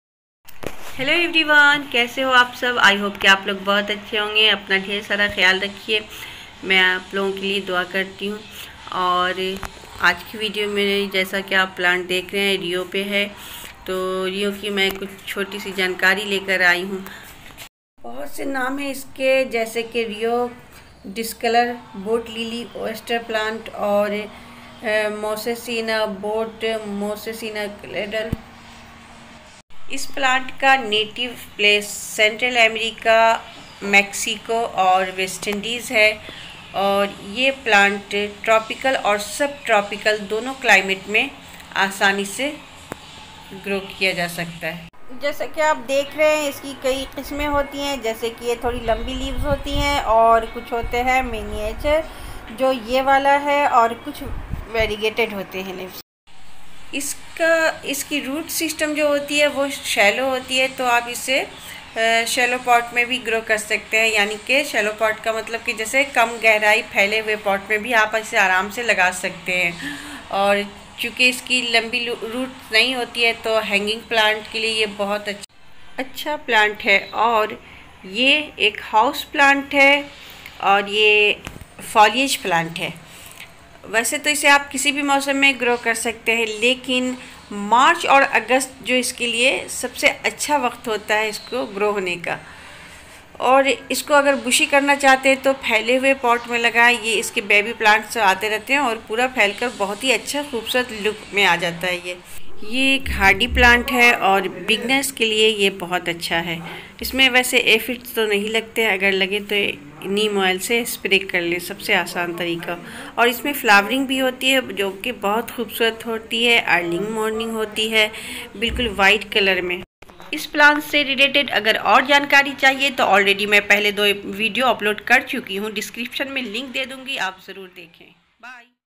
Rhoeo discolor/boatlily/oyester/Moses in the cradle/men in a boat/
Tradescantia spathacea, the boatlily or Moses-in-the-cradle, is a herb in the Commelinaceae family first described in 1788. It is native to Belize, Guatemala, and southern Mexico but widely cultivated as an ornamental and naturalized in parts of Florida, Texas, Hawaii, and various oceanic islands.